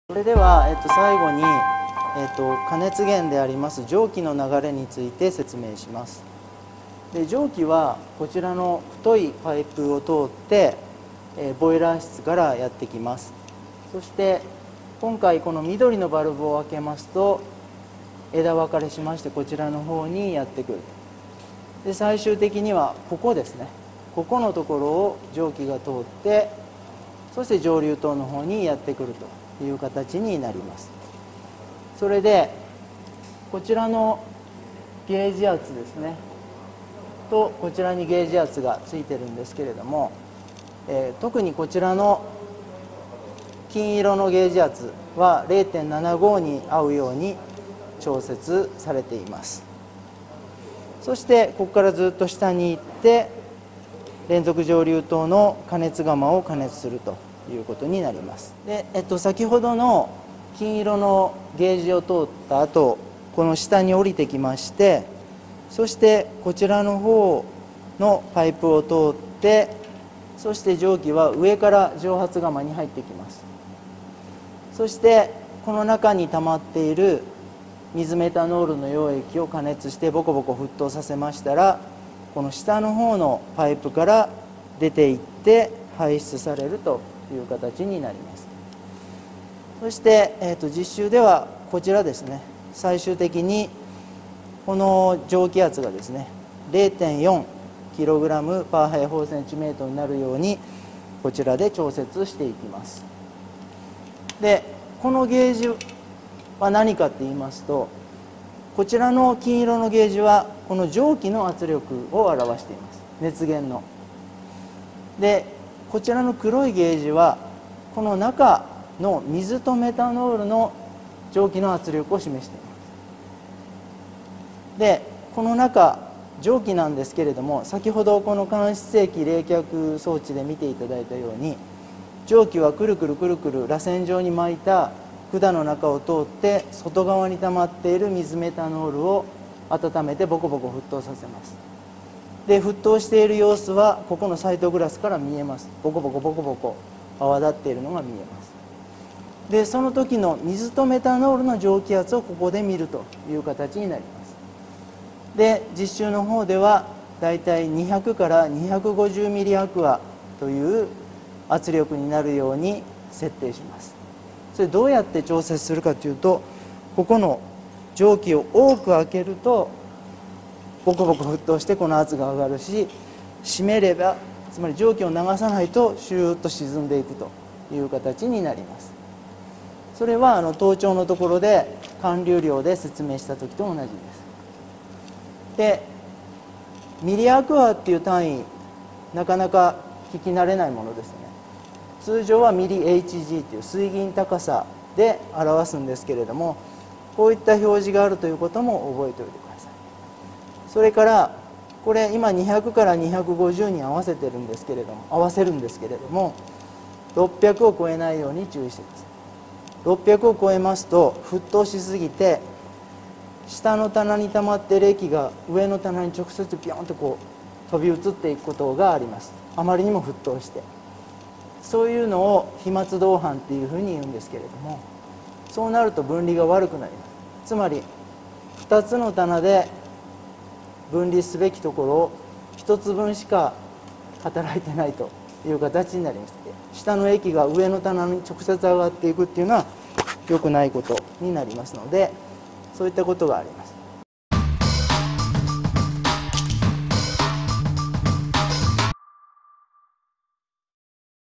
(Eng sub) 連続蒸留装置 蒸気の流れ (Continuous Distillation Equipment-Flow of steam-)
Training of Production and Process Engineering (Continuous Distillation Theory  -Flow of steam-)

Steam is not considered in continuous distillation theory, but it is essential for actual operation.
Steam comes from the boiler room of the Main Research Building.
This is the heat source.
Heating can be adjusted by opening and closing one valve.
Since there is a time lag between heating (opening and closing the valve) and can pressure, it is necessary to adjust the heat source little by little, which requires technology.
It is important that the can pressure is expressed in the unfamiliar unit of "mmAq" .
生産プロセス工学実習（連続蒸留理論 蒸気の流れ）

蒸気は連続蒸留理論では考慮されることはありませんが、実際の運転には必須です。
蒸気は管理研究棟のボイラー室から来ています。
これが熱源となります。
加熱はバルブ1つの開け閉めで調節できます。
加熱（バルブの開け閉め）と缶圧力にはタイムラグが生じるため、熱源の調節は少しずつ行う必要があり技術が求められます。
缶圧力がmmAqと馴染みのない単位で注意が必要です。

生産プロセス工学実習のコースはこちら

解説：岸村 栄毅 （北海道大学 水産学部）
撮影協力・YouTube説明文：熊谷 祐也（北海道大学 水産学部）
動画撮影・編集：北海道大学 バランスドオーシャン運用部